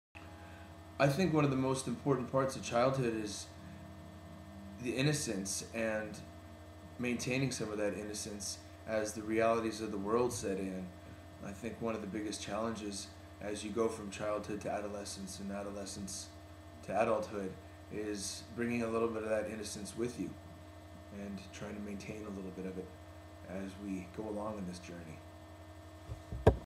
Jonas - Childhood Matters
Jonas tells War Child why childhoodmatters.